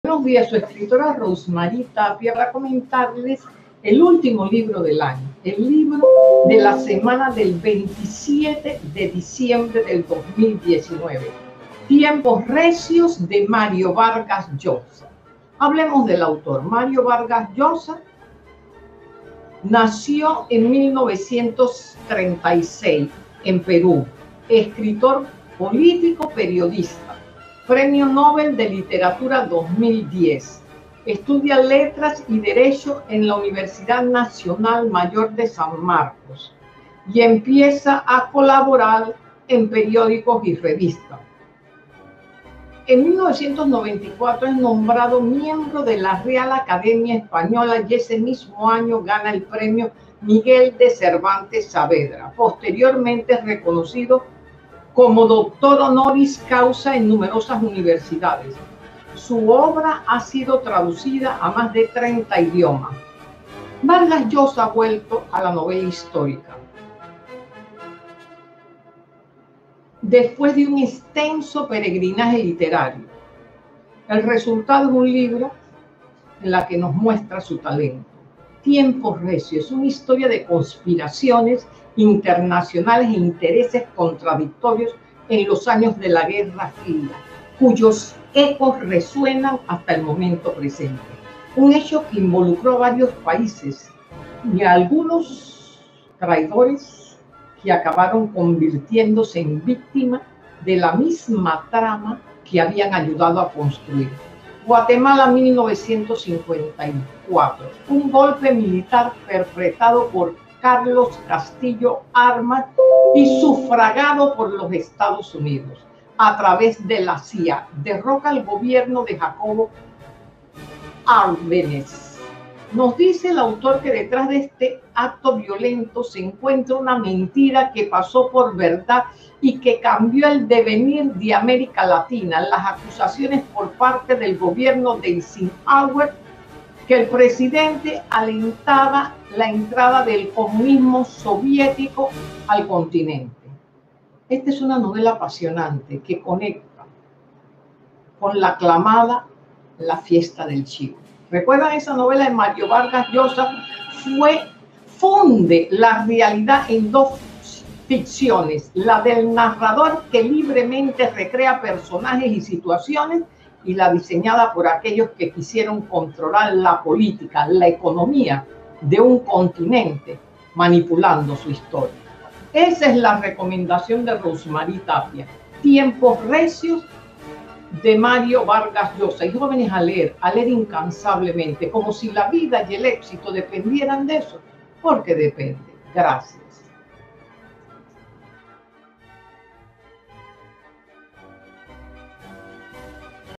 El libro de la semana del 27 de diciembre de 2019 Tiempos recios
El libro de la semana del 27 de diciembre de 2019 Tiempos reciosde Mario Vargas Llosa